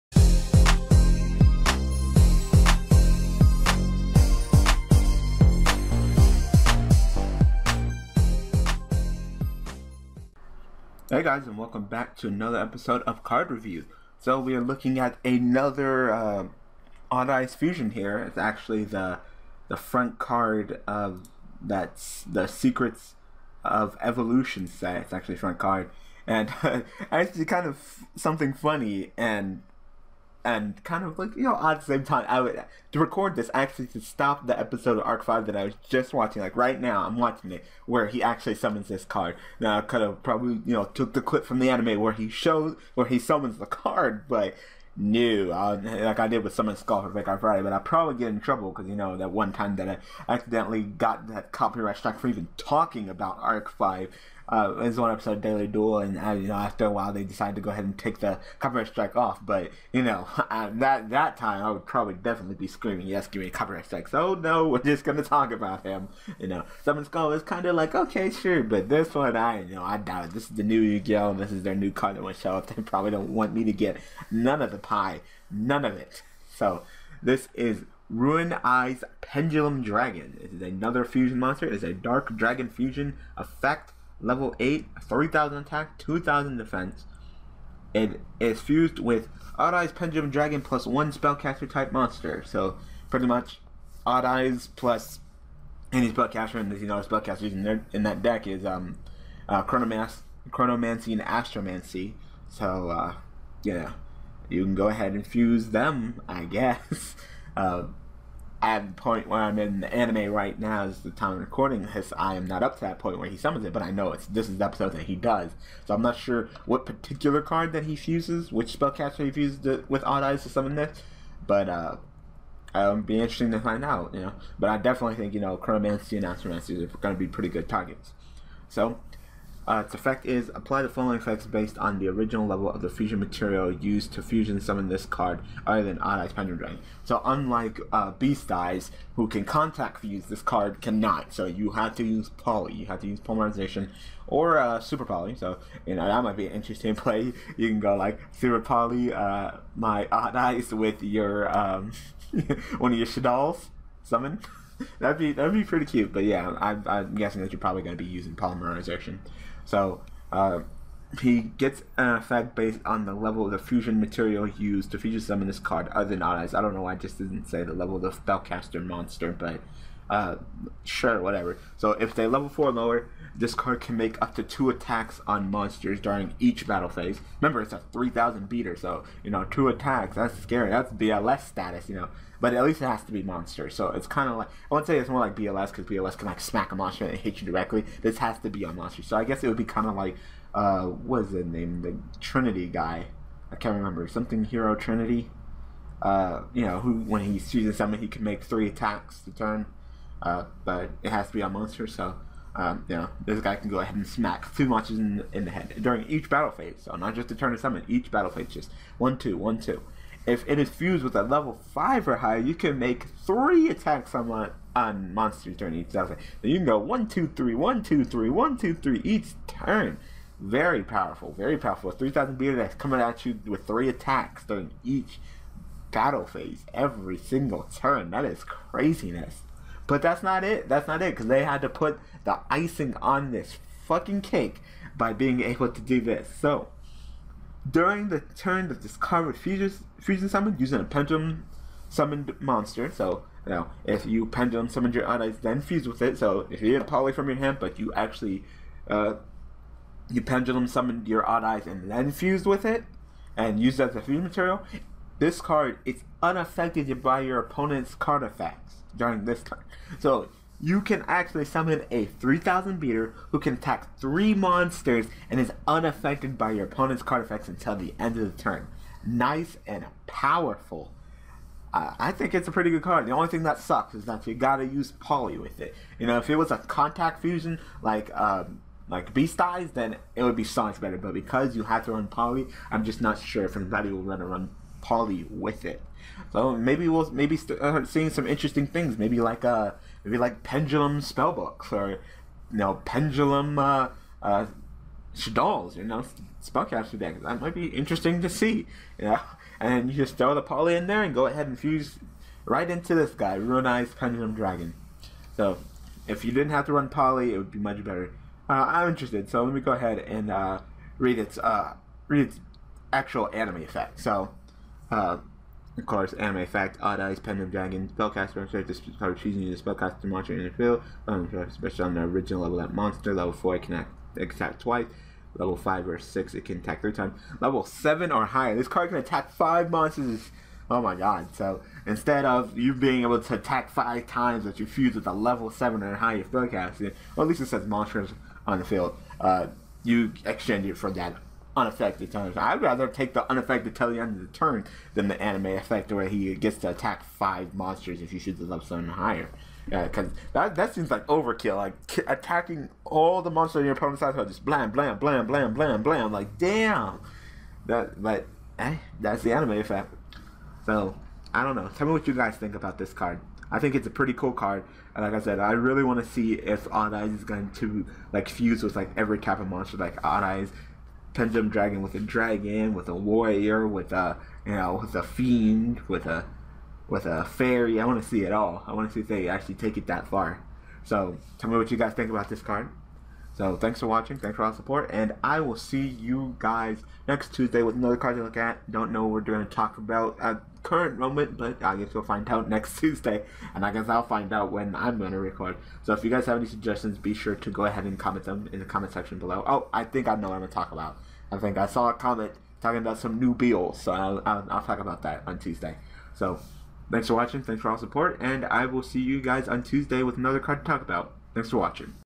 Rune-Eyes Pendulum Dragon: If only it was contact fusion like Beast Eyes is...
Dark / Level 8 / Dragon / Fusion / Effect / 3000 ATK 2000 DEF

"Odd-Eyes Pendulum Dragon" + 1 Spellcaster-Type monster

Apply the following effect, based on the original Level of the Fusion Material Monster used to Fusion Summon this card, other than "Odd-Eyes Pendulum Dragon".
● Level 4 or lower: This card can make up to 2 attacks on monsters during each Battle Phase.
● Level 5 or higher: This card can make up to 3 attacks on monsters during each Battle Phase.
During the turn this card was Fusion Summoned using a Pendulum Summoned monster(s) as a Fusion Material, it is unaffected by your opponent's card effects.